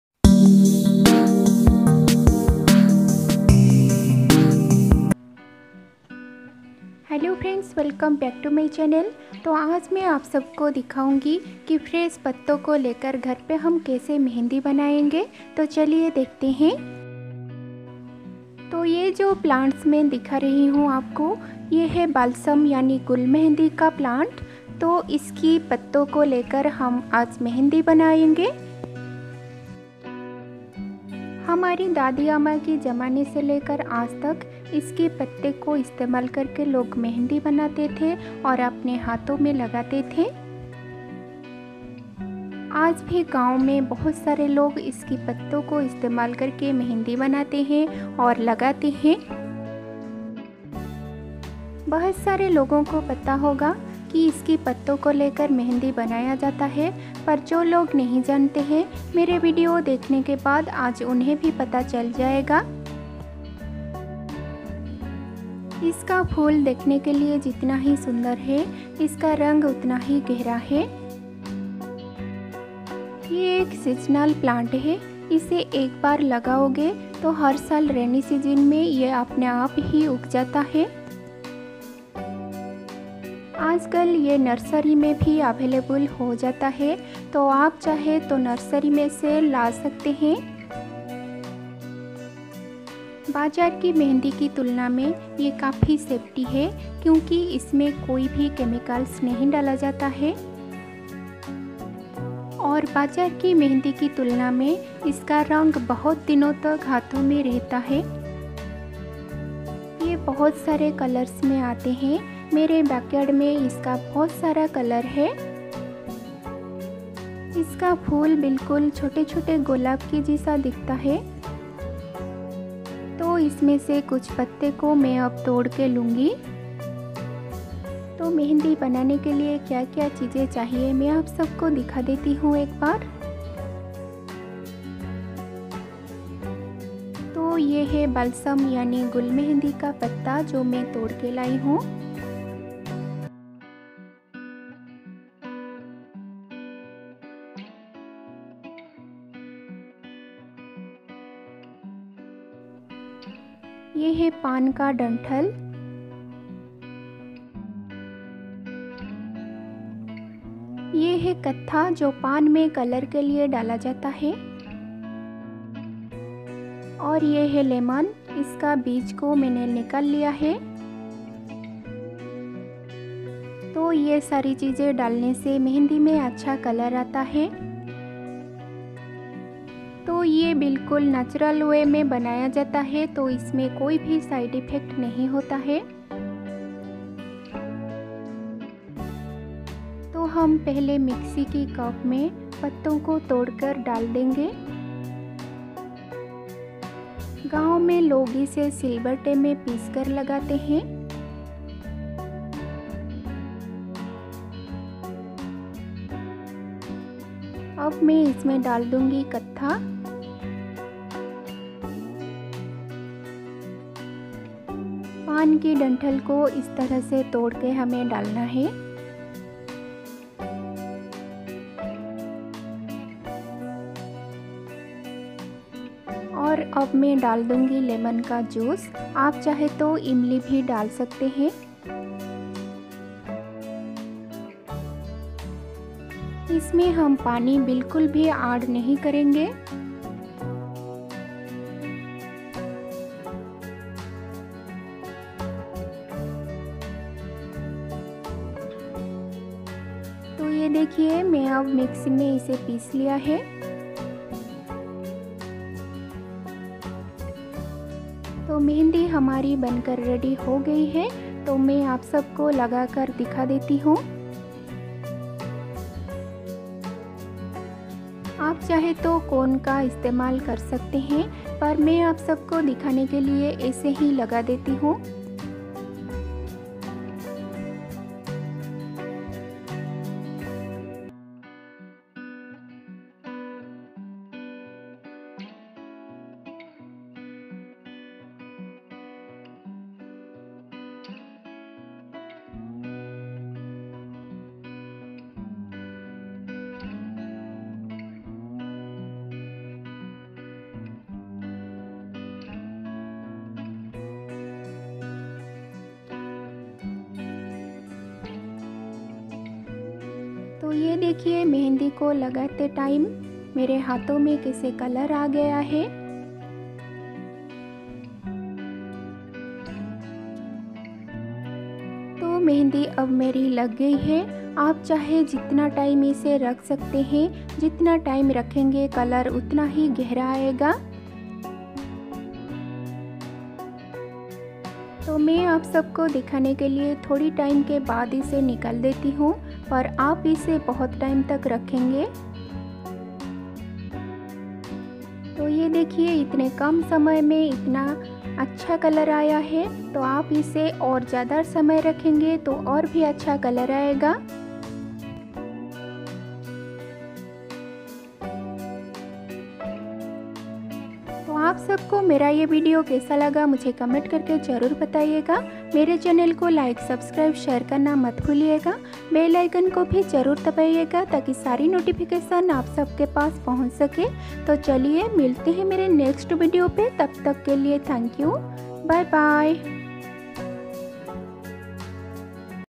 DIY mehndi Made from Fresh Balsam (Gulmehndi) leaves I Home made mehndi l Sovana's daily vlog
Today's vedio -

Things needed-

Balsam (Gulmehndi)leaves
Katha
Pan ki danthal
Lemon juice

Hello everyone. I am Sovana from Odisha.